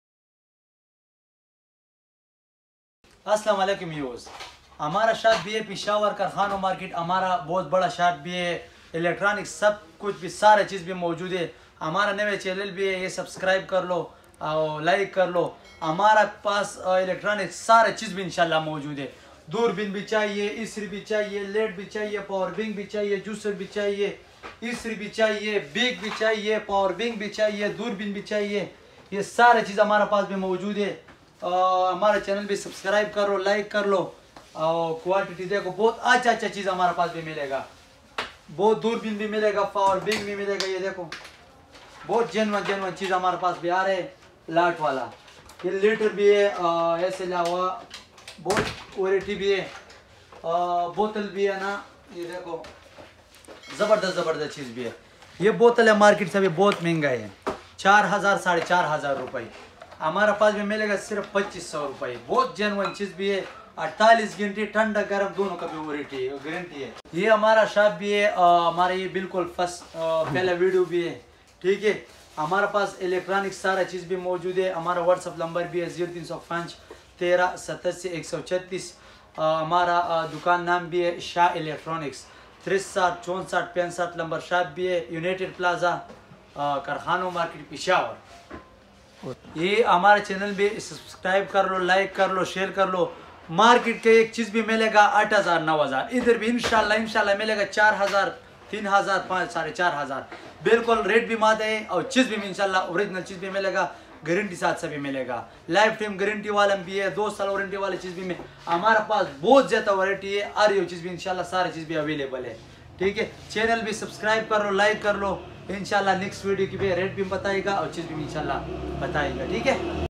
My first Intro Video | My First Vlogs | Electronics Wholesale Shop | Cheap Electronics Items
AoA guys today is my first video on youtube in this channel. In My Channel You  will find electronics, crockery machinery and many other household items Oven, gas stove, laat maal electronics, japani electronics, laat maal car washer and much more. so please watch full video and like share subscribe my channel.